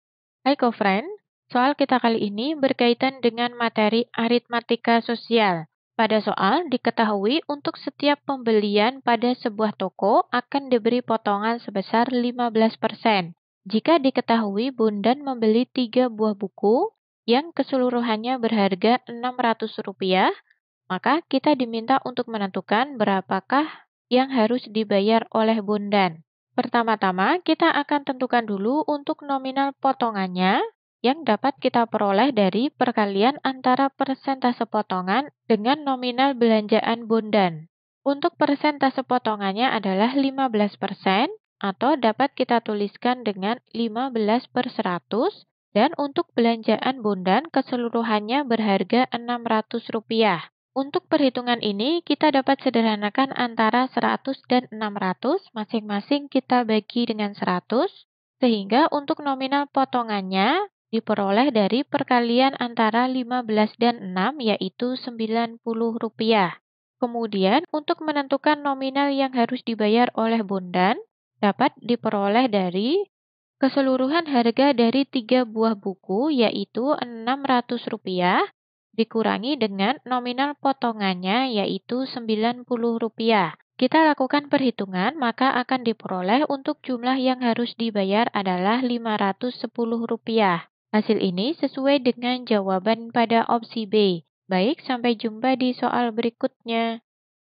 Pada sebuah toko, setiap pembelian akan diberi potongan 15 %. Bondan membeli 3 buah buku yang kes...
Pada sebuah toko, setiap pembelian akan diberi potongan 15 %. Bondan membeli 3 buah buku yang keseluruhannya berharga Rp600,00. Maka Bondan harus membayar:

Topik atau Materi: Rabat (Diskon), Bruto, Tara, dan Neto - ARITMETIKA SOSIAL (APLIKASI ALJABAR) - ALJABAR - Matematika Kelas 7 SMP - KTSP

Sumber Soal: Bimbingan Pemantapan Matematika SMP, KTSP, RS, 2008 , Yrama Widya

00:00 : Bedah Soal
01:00 : Konsep, rumus dan pengertian Rabat (Diskon), Bruto, Tara, dan Neto
01:18 : Langkah penyelesaian soal
00:37 : Jawaban akhir
01:07 : Penutup